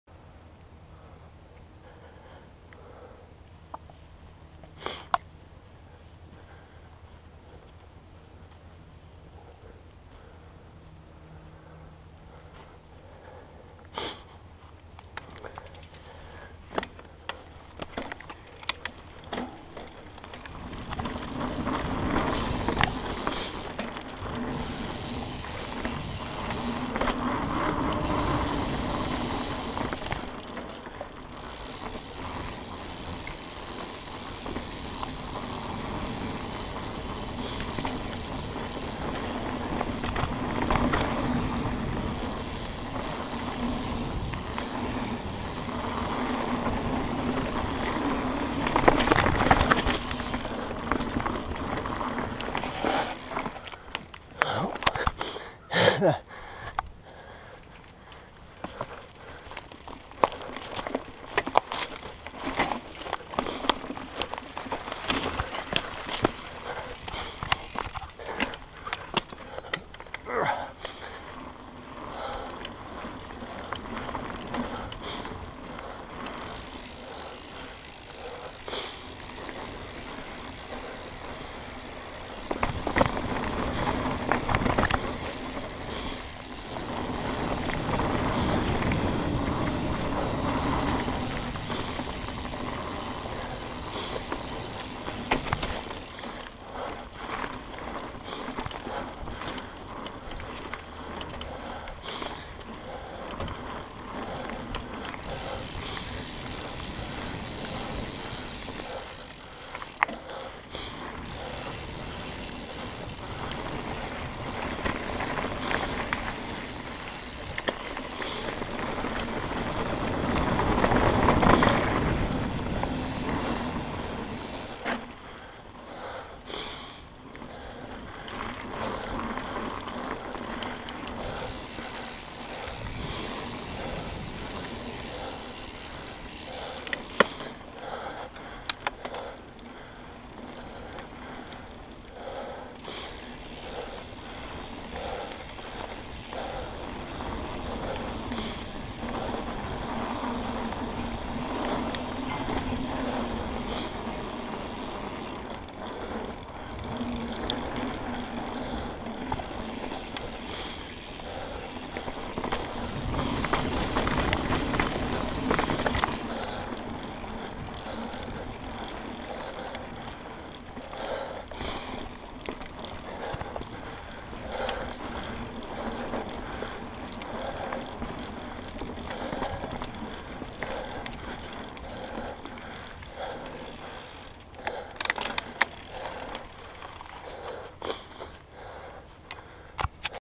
Jake's Breakaway - MTB - Black Diamond single-track ♦ Hydrocut Video Map
Jake's Breakaway
Difficulty - Black Diamond
Length - 450m

This is a nice flowing section with some rocks to navigate along the way. Have to go off path to avoid a downed tree. Hurricane Dorian is nowhere near us... but we still got a decent storm. 3 trails had trees down today, this was the first.